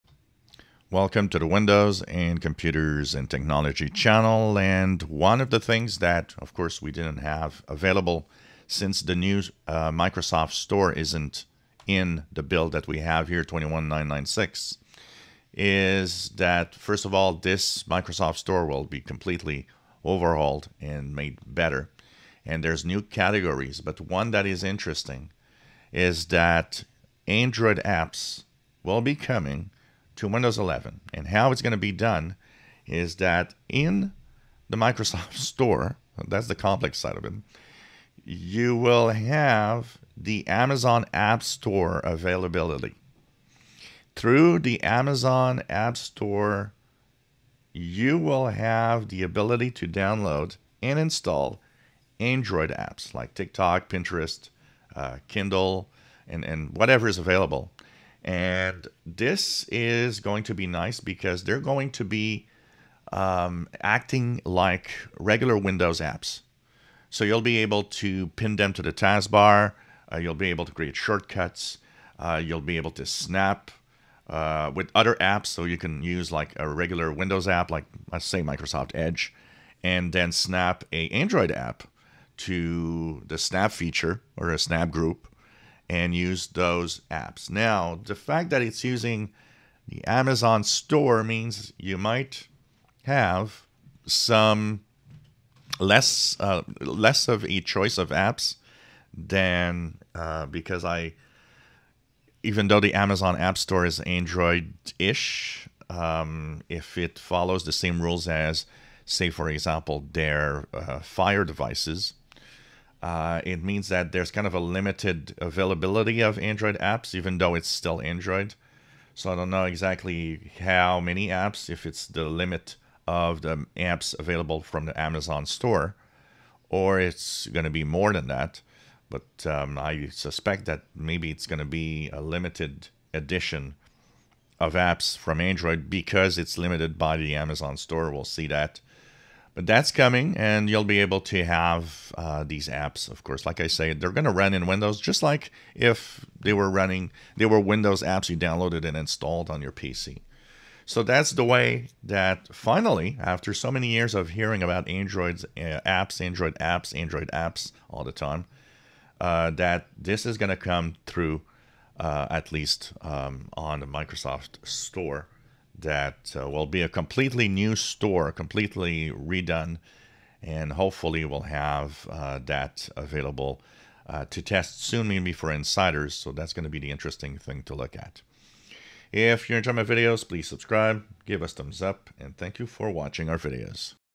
Windows 11 Android apps will come from Amazon store from Microsoft Store
Finally Android apps in Windows 11, but it might be limited due to the use of the Amazon store